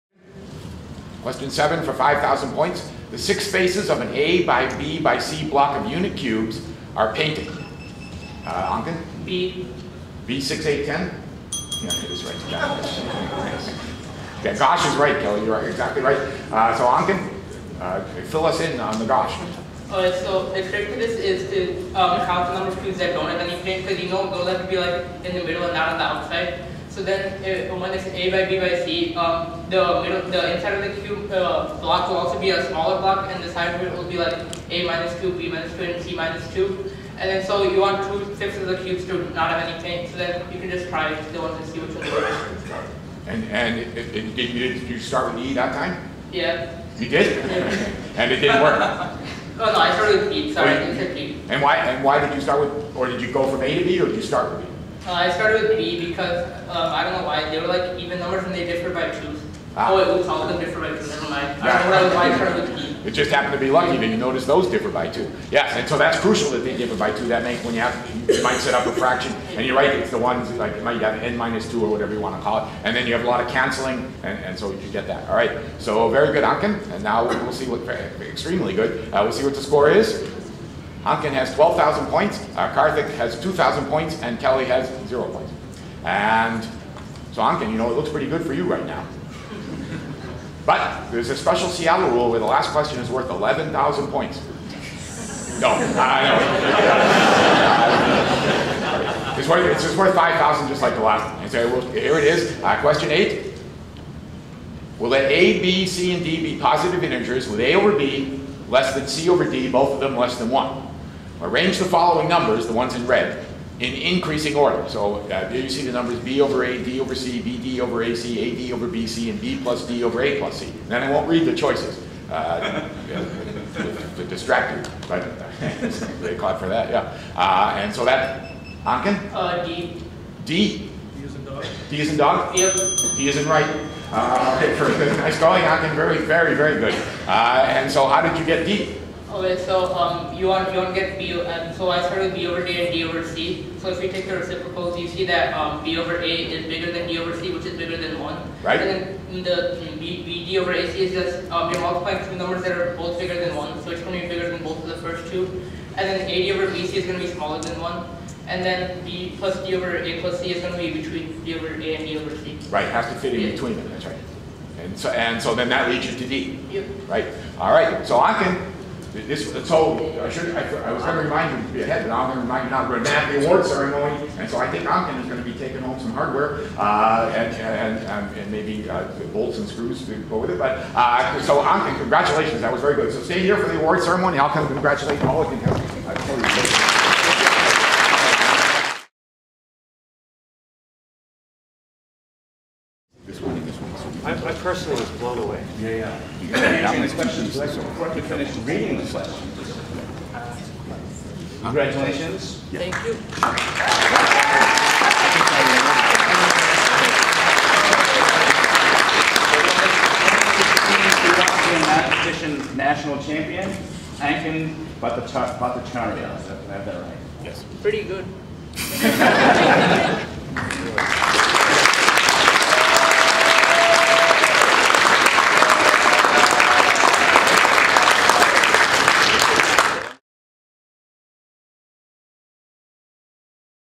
WWTBAM JMM 2016 - Finals Part 4
Ankan continues to roll on the last two questions of the finals of the 2016 national Who Wants to Be a Mathematician